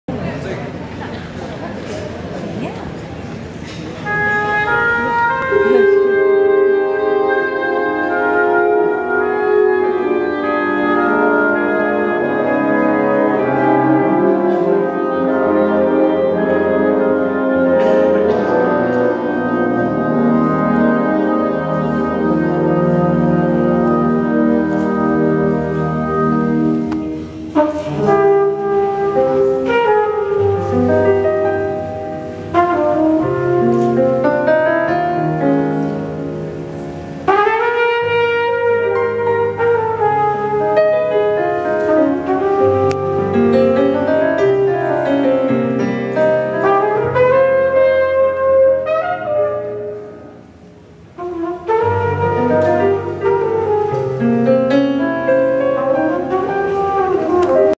Bugel solo Mariniers Kapel der Koninklijke Marine in Zuidland
Op 16 2016 september was er een jubileumconcert van de Oranjevereniging Zuidland omdat die 80 jaar bestond, met daarbij De Mariniers Kapel der Koninklijke Marine, Humphrey Campbell en Caesar Zuiderwijk.